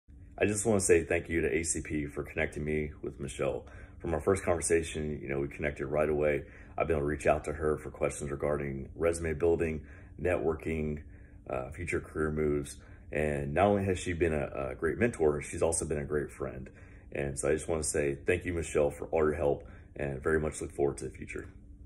Jordon H. Thanks His Mentor Michelle from In-N-Out
Jordon served in the U.S. Coast Guard until 2016 and has worked with his mentor Michelle, a Marketing Director at In-N-Out, on exploring different job opportunities and eventually helping him land a great job. Both based in Southern California, Michelle and Jordon had the chance to meet in person several times for their mentoring sessions.